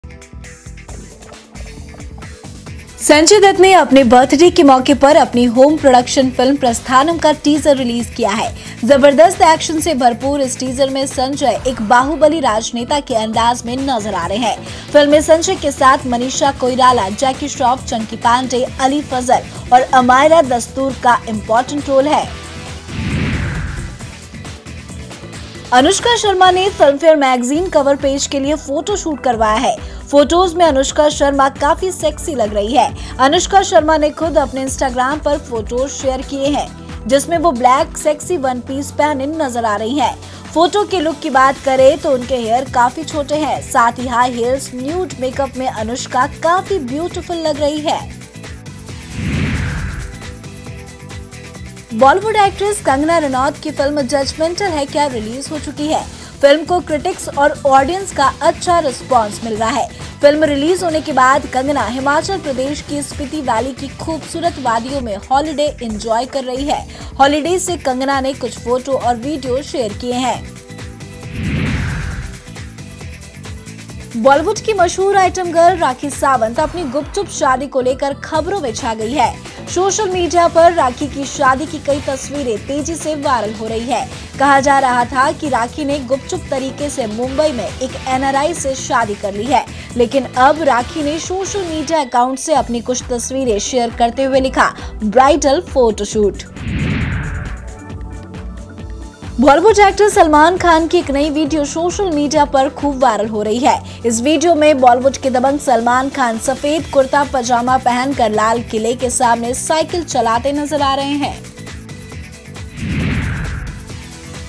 लाल किले के सामने सलमान ये क्या कर रहे हैं..??
संजय दत्त की फिल्म प्रस्थानम का टीज़र रिलीज.. राखी सावंत ने करवाया ब्राइडल फोटोशूट...देखिये BOLLYWOOD LATEST UPDATE ‘BOLLYWOOD BREAK ’ में..